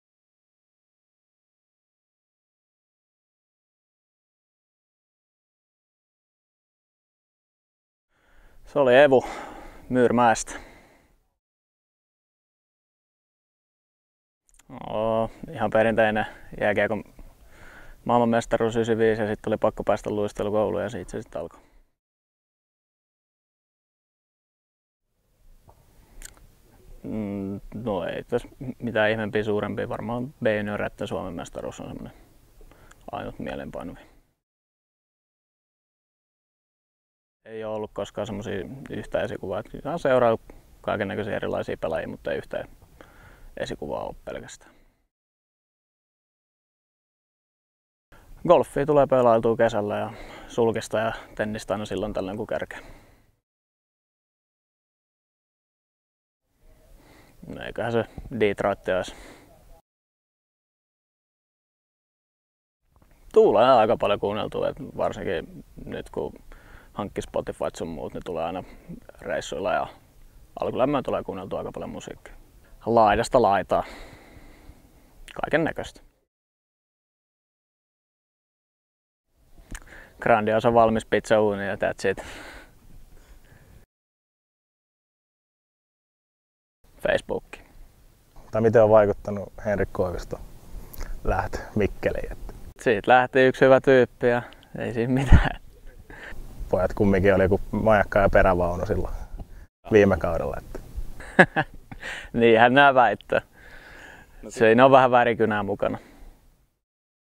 Toni Pulkkinen #12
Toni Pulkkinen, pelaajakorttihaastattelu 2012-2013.
Haastattelu, kuvaus ja editointi: Janne Tikkaoja